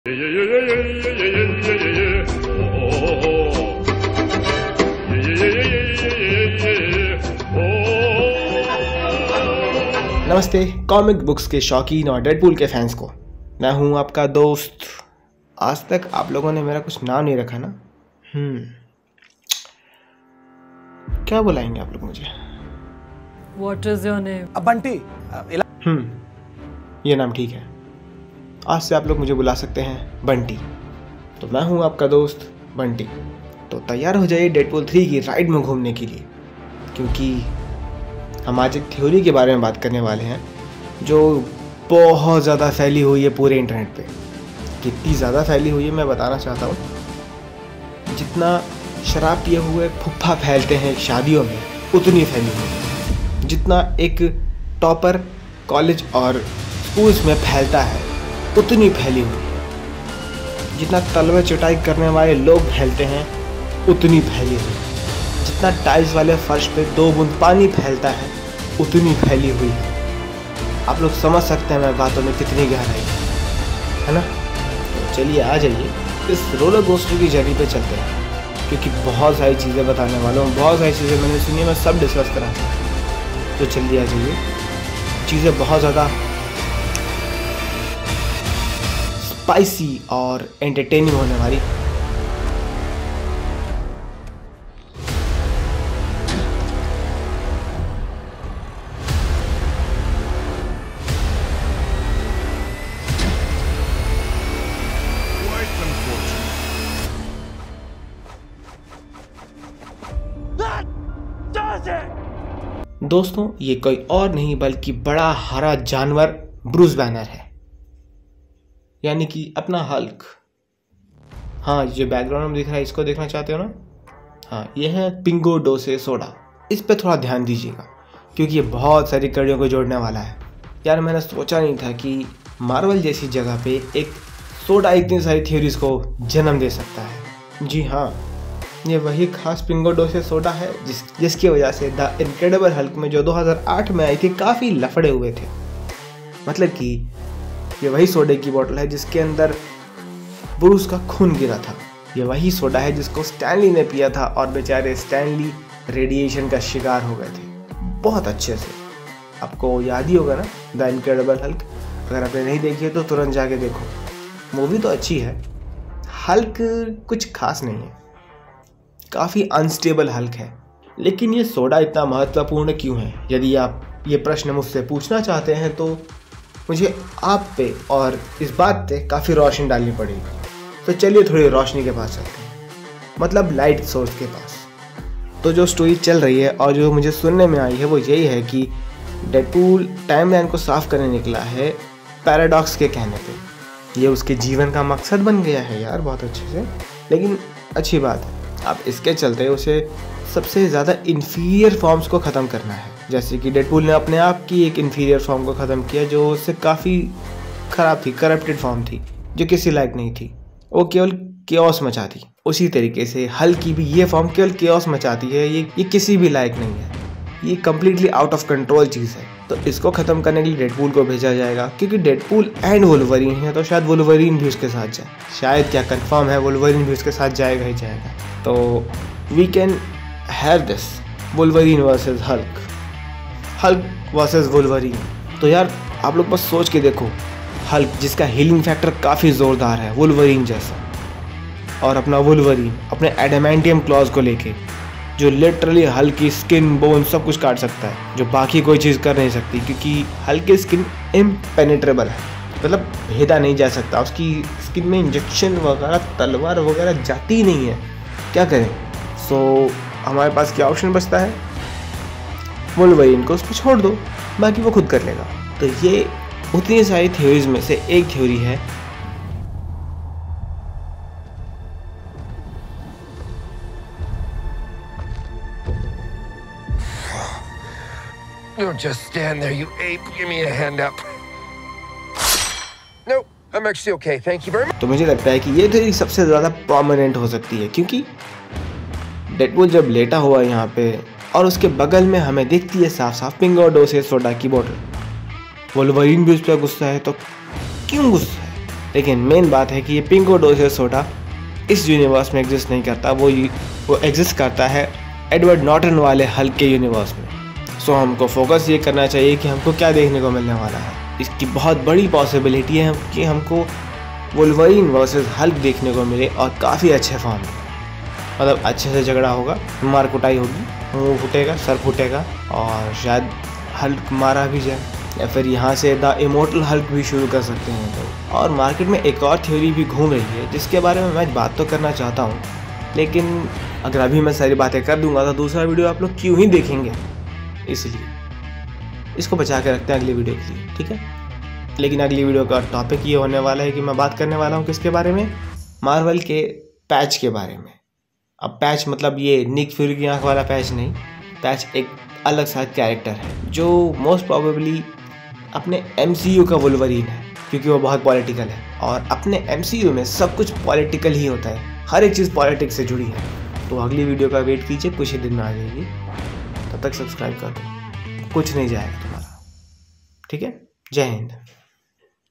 "🔥 Unleashing Chaos: Deadpool vs. Wolverine vs. Hulk! 🦸‍♂️💥 The Ultimate Showdown Begins!"
🎬 Get ready for an adrenaline-packed rollercoaster ride through the multiverse as Bunty, your ultimate guide to all things comic book, unveils a mind-bending theory that's set to redefine superhero cinema! 🦸‍♂️🍿

In this gripping exploration, we delve into the tantalizing world of Deadpool 3 speculation, where every frame is packed with excitement, Easter eggs, and jaw-dropping revelations. From the iconic showdown between Deadpool and Wolverine to the epic clash with the Hulk from 2008's cinematic universe, no stone is left unturned in this thrilling narrative.

Join Bunty as he dissects the intricate web of connections between Deadpool, Wolverine, and the infamous Pingo Doce soda, unraveling a theory that's been setting the internet ablaze! With heart-pounding action, witty banter, and enough geeky references to satisfy even the most die-hard fans, this is one theory you won't want to miss.

So, grab your popcorn, strap in, and prepare for a journey that transcends time, space, and the very fabric of reality itself. Because when Deadpool is on a mission to clean up the timeline, anything can happen – and trust us, you'll want a front-row seat for all the chaos and mayhem! 🚀🔥